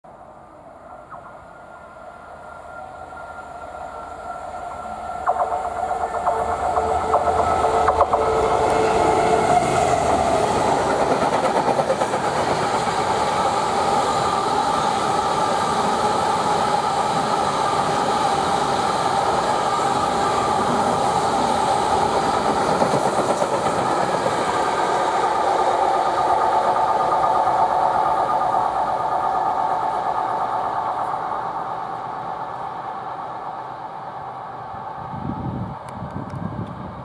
Freightliner 70003 with Westbury to Margam empty ballast wagons
70003 passes through Keynsham with Westbury to Margam empty ballast wagons. 11/7/2015